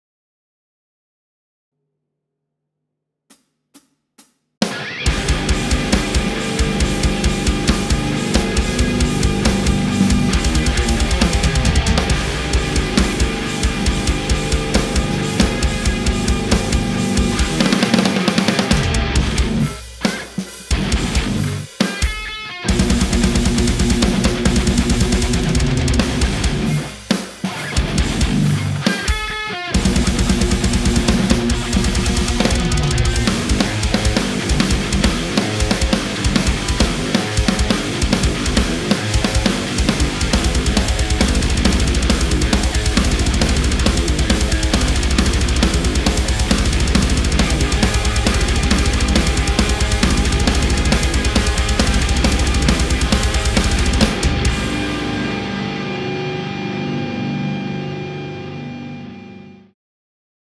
Sunday with Ola - Riff Challenge #29 - Heikenen3000
My 2nd participation in the Riff Challenge - Enjoy!

Drums by Ola Englund
everything else by Heikenen3000.